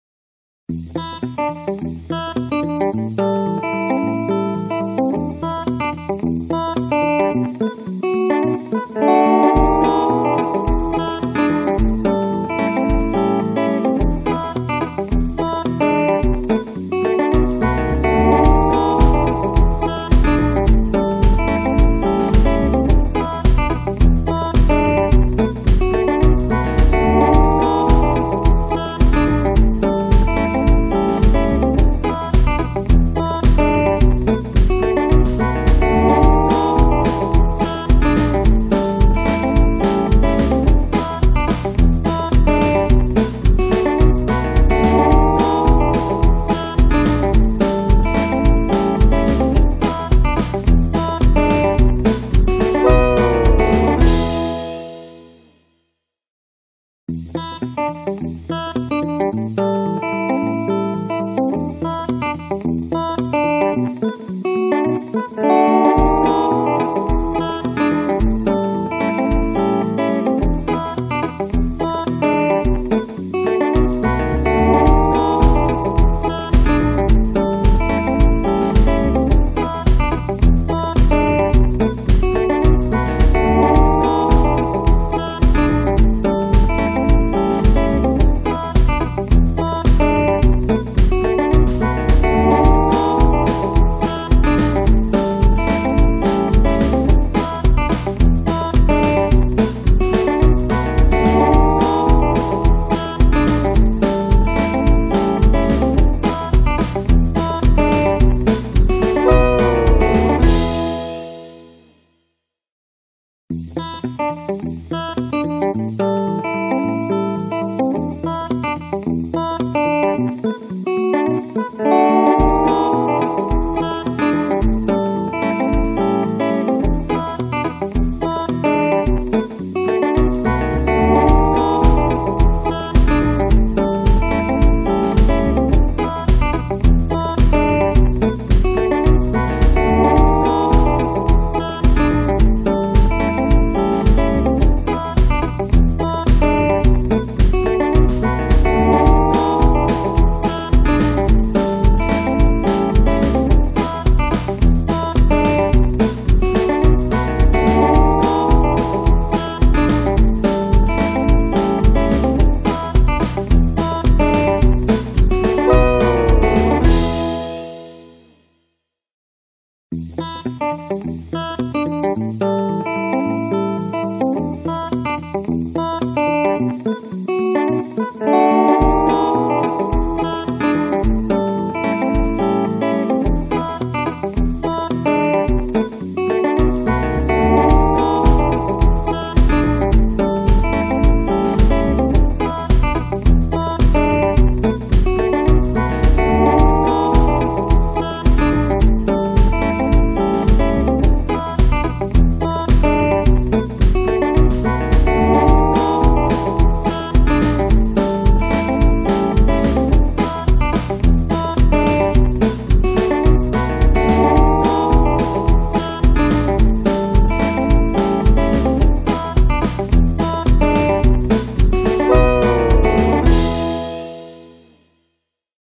Chicken Picking Country Guitar (Free Download Background Music)
Rockabilly country, in the style of Chet Atkins. The other instruments are bass and drums. Authentic country chicken picking guitar sound. This track will work great for any project needing a country rockabilly flair.